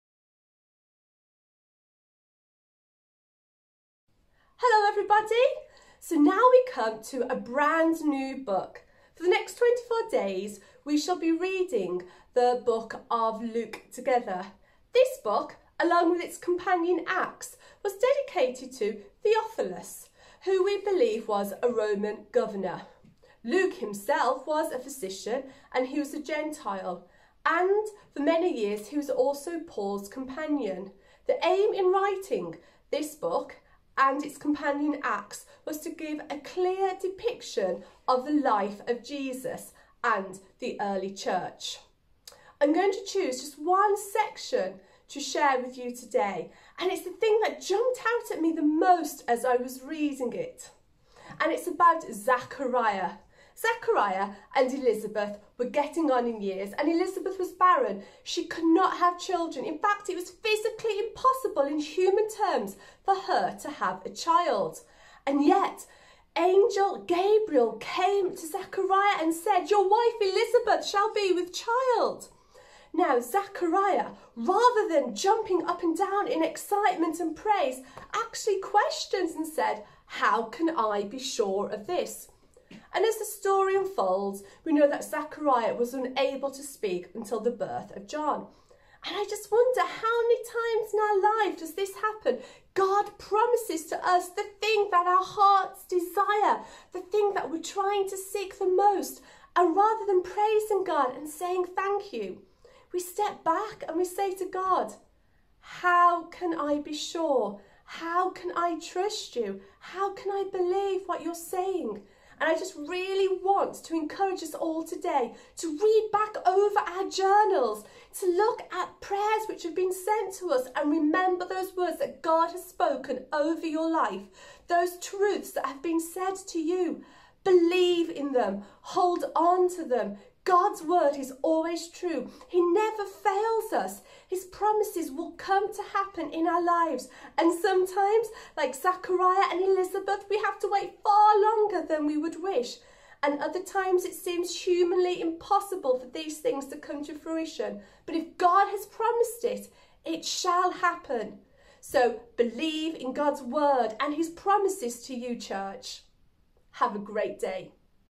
Luke 1: Connect Life - Kirsty
Today Kirsty take us to our new book of the Bible we are going to go through together. Luke's Gospel and asks us whether we have forgot to thank God for his promises.